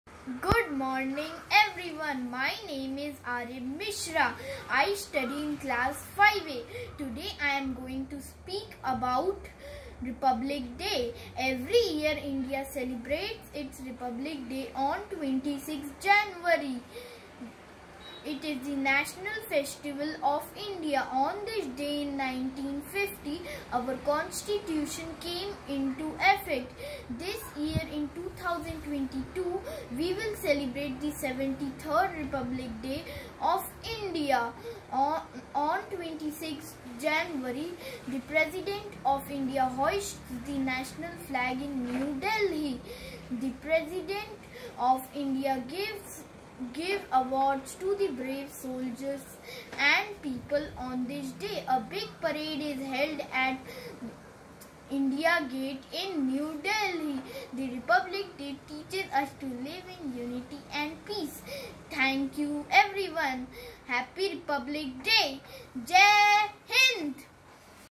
Speech on Republic Day By Aariv Mishra 🇮🇳🇮🇳🇮🇳
Speech on Republic Day## By Aariv Mishra ## 8 Lines on Republic Day #